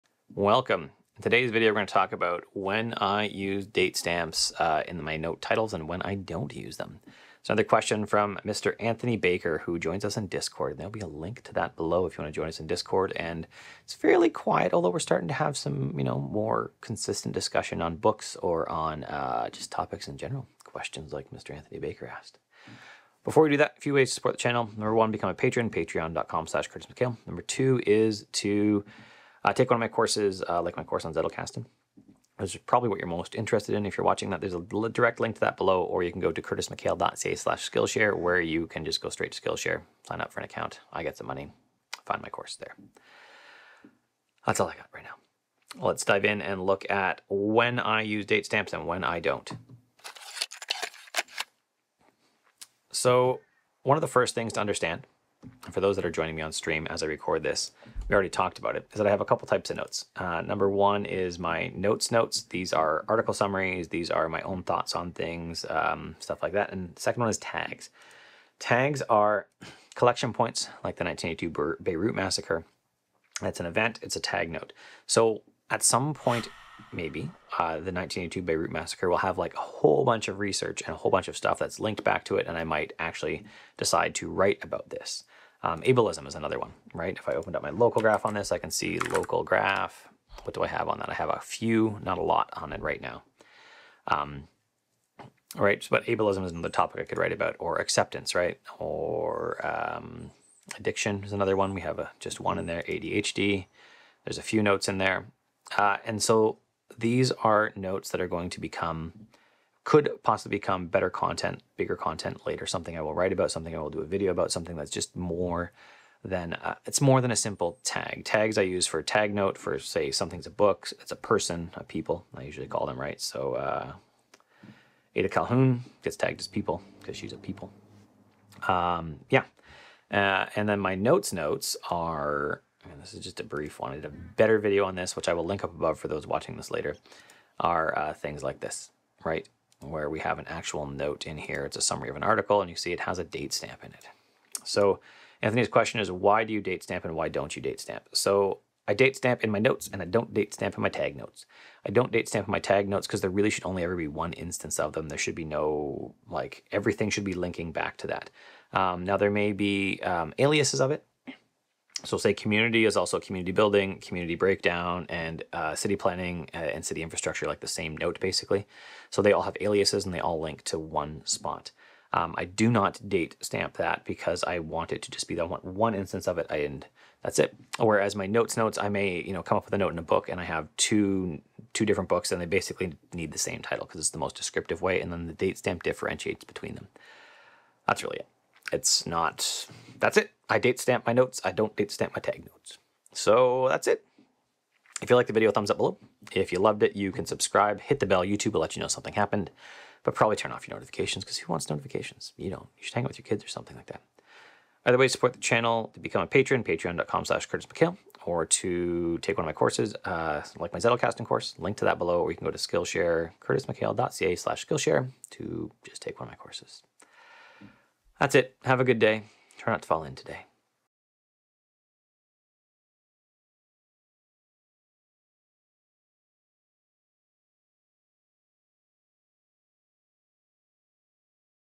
Obsidian Date Stamps
Today we’ll answer another question from Anthony Baker, when do I put datestamps on my notes?

## Links

## Courses

## My Gear